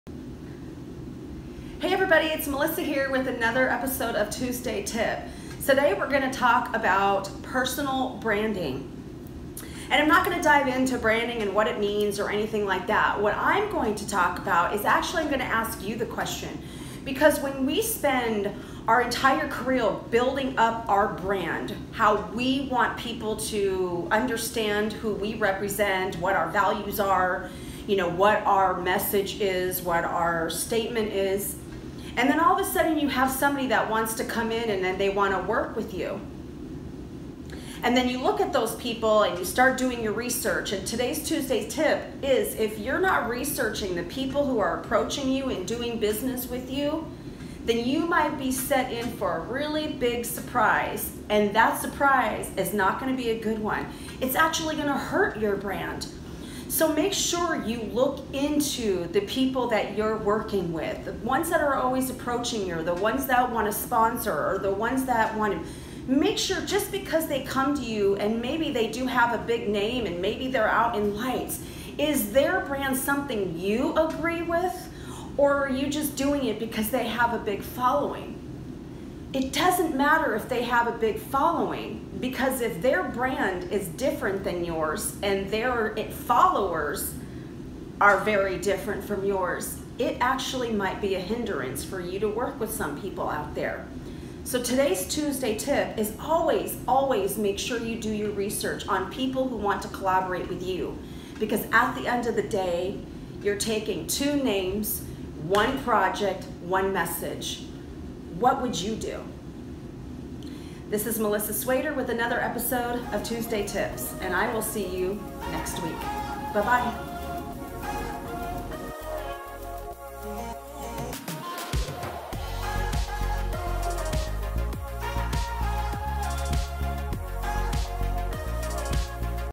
**TUESDAY TIPS** - Episode 13: Research Before you commit to collaboration projects
We all spend countless hours building a reputable name and personal brand for ourselves... But if you don't do "THIS" you may regret your choices!!!  So Be AWARE! We often times get so caught up in the "influencer" marketing because of "followers" that we forget who actually might be following.  So here is a great message before you commit to collaboration projects.  Enjoy!